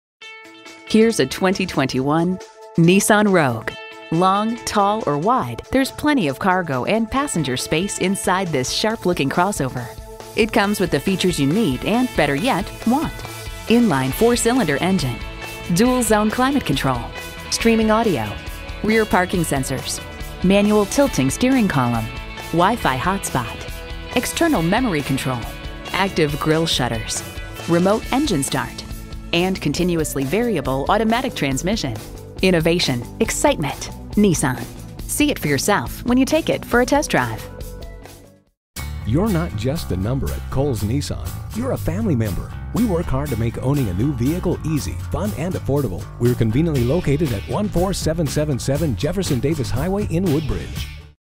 Used 2021 Nissan Rogue Washington DC VA Woodbridge, VA P17252 - SOLD
Year: 2021
Make: Nissan
Model: Rogue
Trim: SV
Engine: 2.5L I4 DOHC 16V
Transmission: CVT with Xtronic
Color: Gray
Interior: Charcoal
Mileage: 9488
Stock #: P17252
VIN: 5N1AT3BB7MC680927
Certified. Gun Metallic 2021 Nissan Rogue SV AWD CVT with Xtronic 2.5L I4 DOHC 16V AWD.brbrCERTIFIED PRE-OWNED 7YR 100K MILE WARRANTY. Recent Arrival! 25/32 City/Highway MPGbrbrNissan Certified Pre-Owned Details:brbr * Vehicle Historybr * Includes Car Rental and Trip Interruption Reimbursementbr * Warranty Deductible: $100br * Transferable Warrantybr * Roadside Assistancebr * Limited Warranty: 84 Month/100,000 Mile (whichever comes first) from original in-service datebr * 167 Point InspectionbrbrbrAuto Giants Nissan! Home of "The Giant Plan"

Address:
14777 Jefferson David Hwy